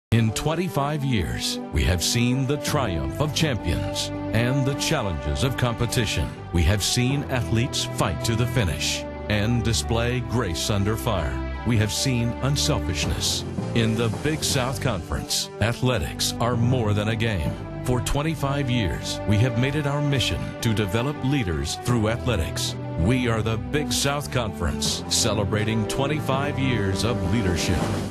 Big South Conference: Leadership
The Big South Conference proudly shares its mission of Developing Leaders Through Athletics.  For more on the Big South, visit us